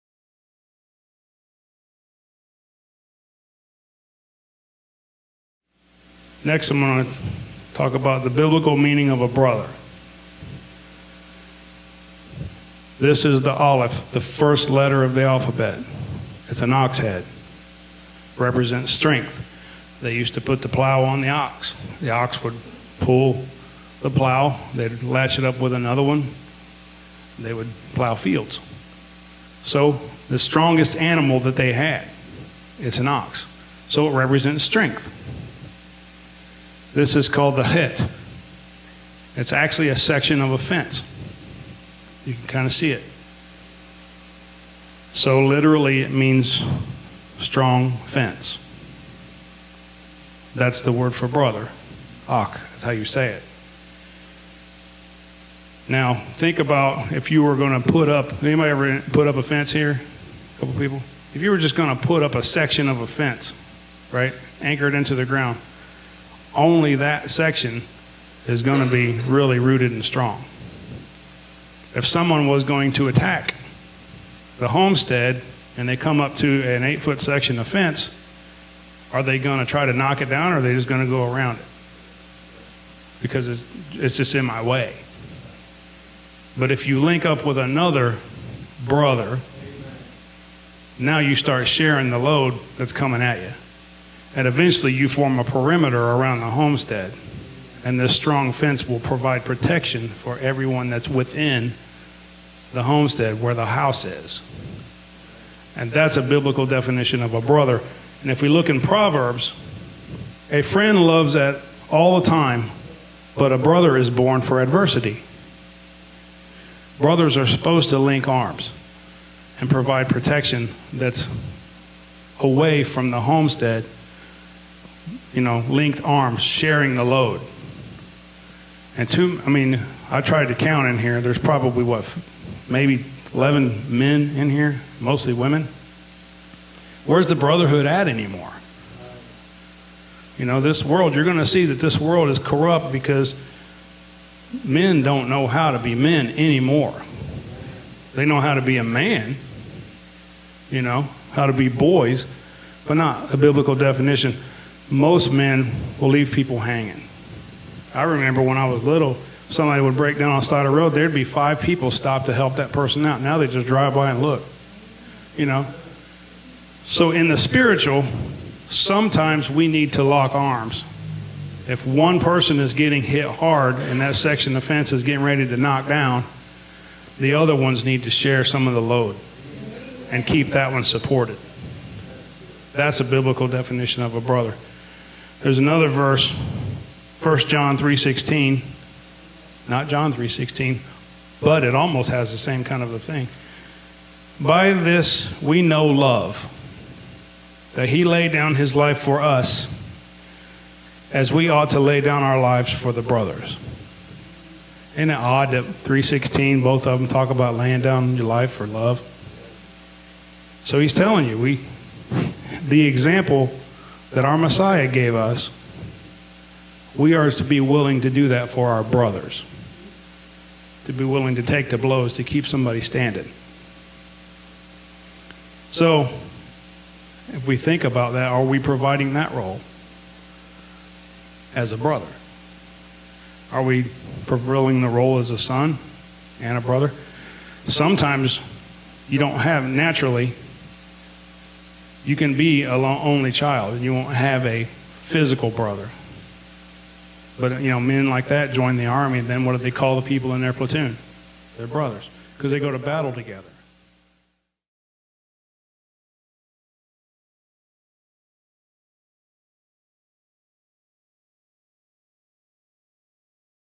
Biblical Definition of a Brother
What does the Bible say a Brother is?  How does God look at us and judge us that we are Brothers in HIM?  What is the role of a Brother?